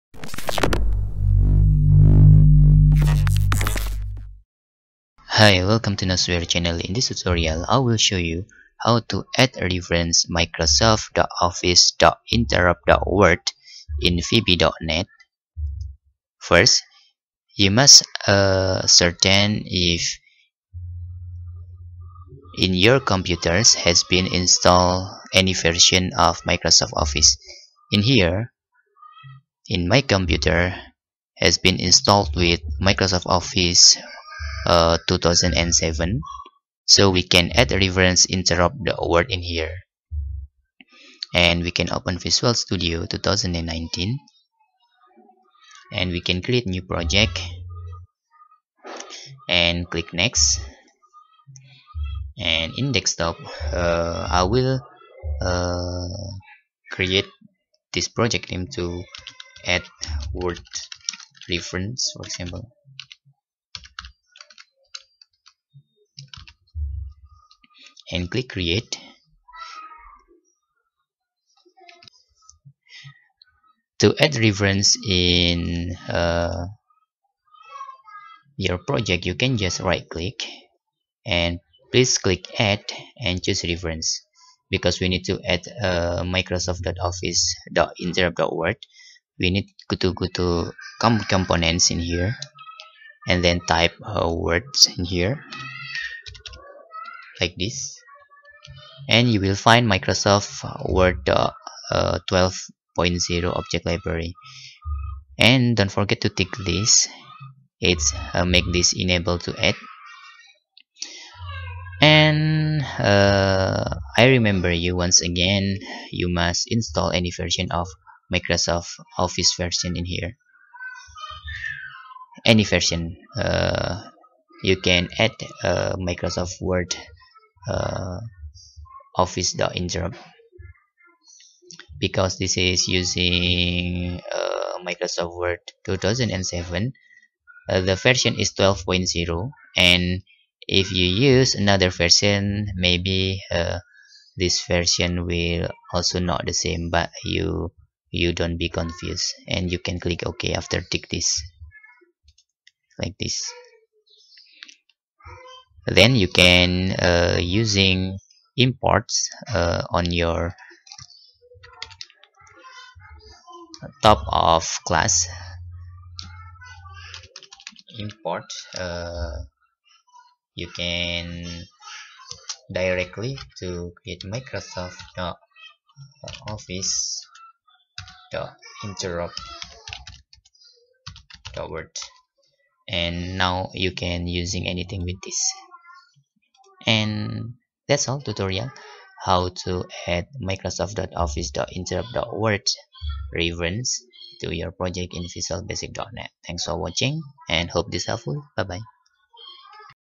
How to Add Microsoft.Office.Interop.Word in Visual Studio
This video will guide You how to add Microsoft.Office.Interop.Word in Visual Studio